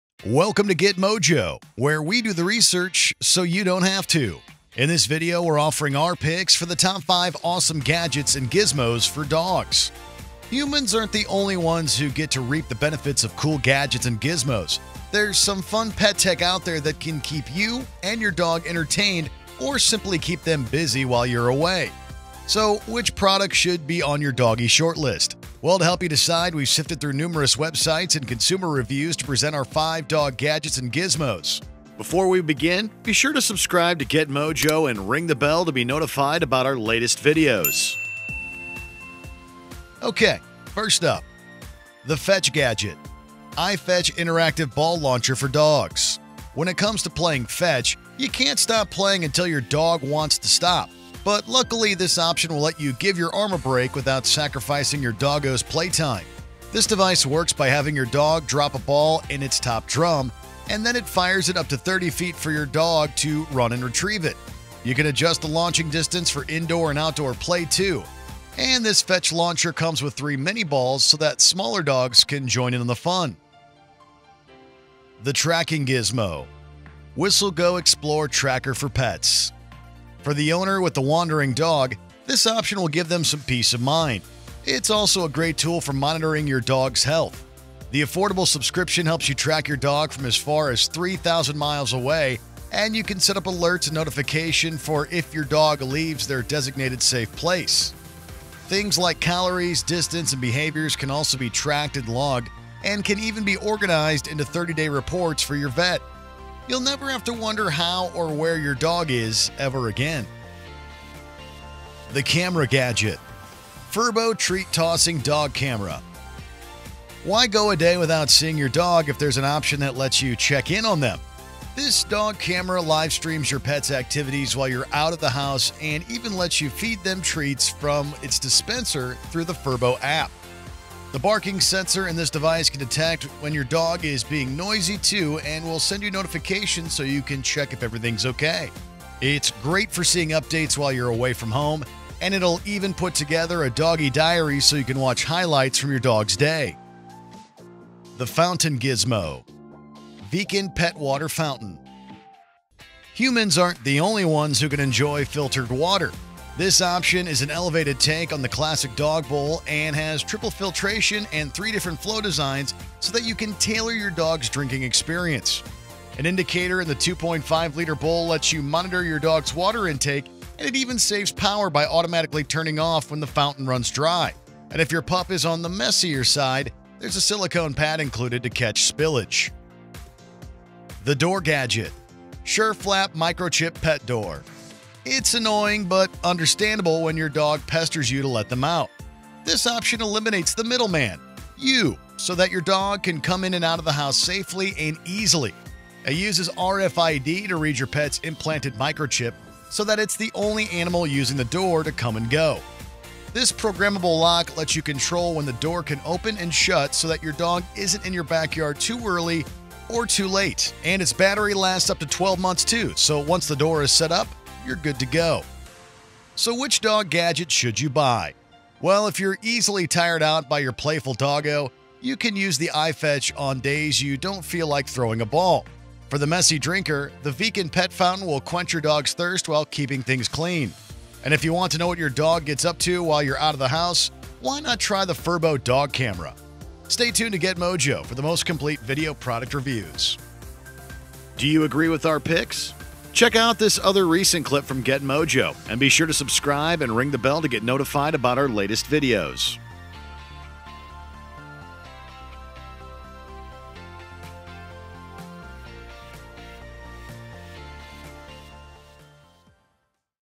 Top 5 Awesome Gadgets and Gizmos for Dogs
Humans aren’t the only ones who get to reap the benefits of cool gadgets and gizmos. There’s some fun pet tech out there that can keep you and your dog entertained, or simply keep them busy while you’re away. So, which product should be on your doggy shortlist? Well, to help you decide, we’ve sifted through numerous websites and consumer reviews to present our 5 dog gadgets and gizmos.